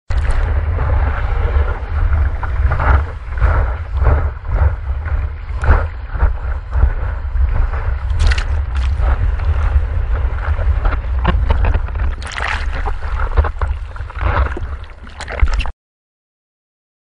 Trace: Surfing - Chuma Santiago Garcia at San Lorenzo
Trace auto edited Chuma Santiago Garcia's surfing session at San Lorenzo on Mar 10, 2015.

Waves: 6
Min of Surfing: 65.0
Distance: 173 yd
Calories Burned: 194
Max Speed: 10.8 mph